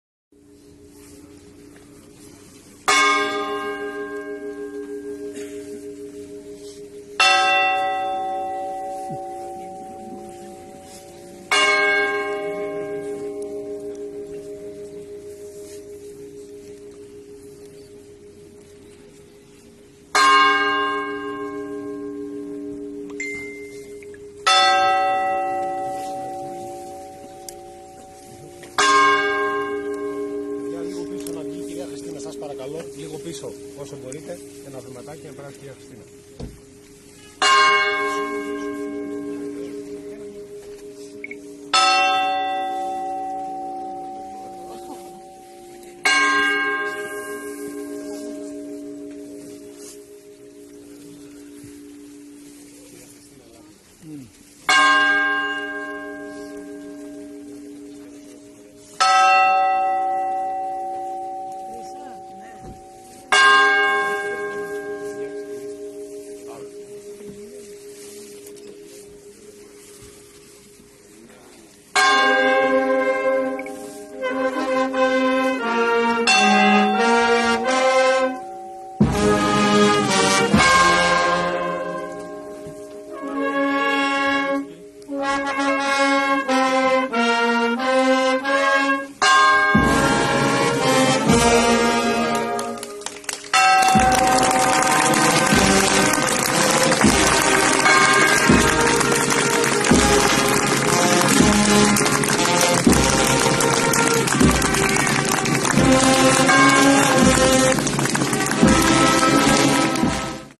Στη γενέτειρά του η σορός του Βασίλη Καρρά
Στον Ιερό Ναό Αγίου Νέστορος στο Κοκκινοχώρι Καβάλας έφτασε η σορός του Βασίλη Καρρά, καθώς σήμερα θα ταφεί ο “άρχοντας” του λαϊκού τραγουδιού.

Χιλιάδες κόσμου, που ολοένα και αυξάνονται, βρέθηκαν από νωρίς το πρωί στην εκκλησία, για να πουν από κοντά το τελευταίο “αντίο” στον Βασίλη Καρρά.